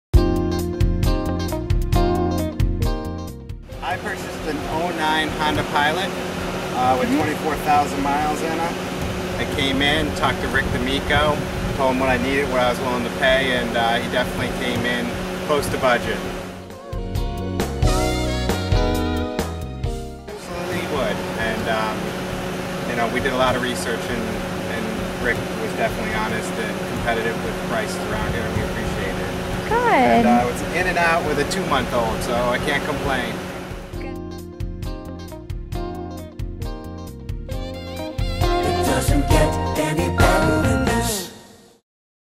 Customer Testimonial : Brian - Honda Pilot - Dick Ide Honda, Rochester NY Dealer Review
Brian picks up his Honda Pilot today from Dick Ide Honda! Here is what Brian he has to say!